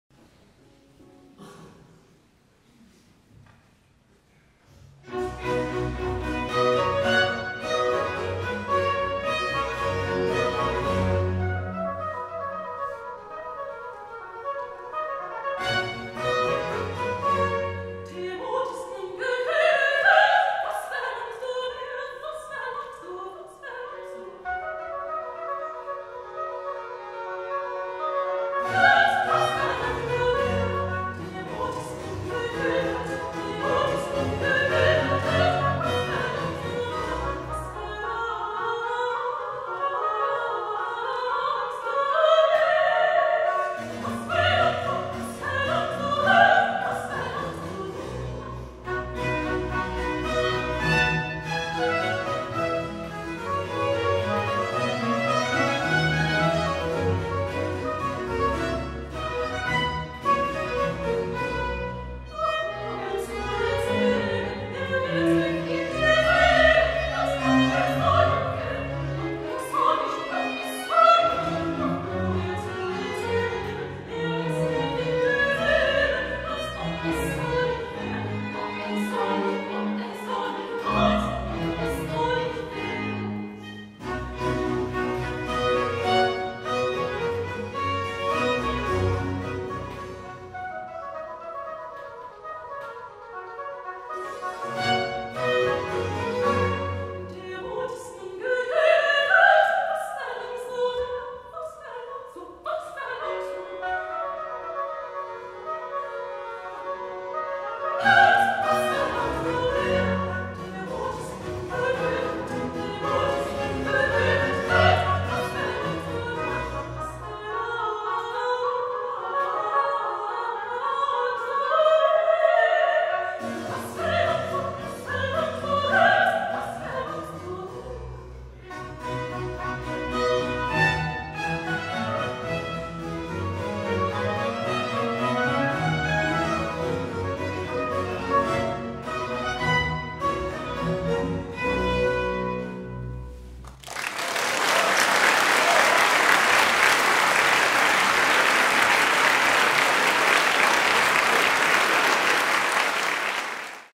L'Escadron Volant de la Reine @ Collégiale d'Herment
L'Escadron Volant de la Reine à la Collégiale d'Herment.

Eugénie Lefebvre • soprano
Vincent Blanchard • hautbois
Maris Rouquié, Josèphe Cottet • violons
Benjamin Lescoat • alto
Antoine Touche • violoncelle
Sanne Deprettere • contrebasse
Clément Geoffroy • orgue et clavecin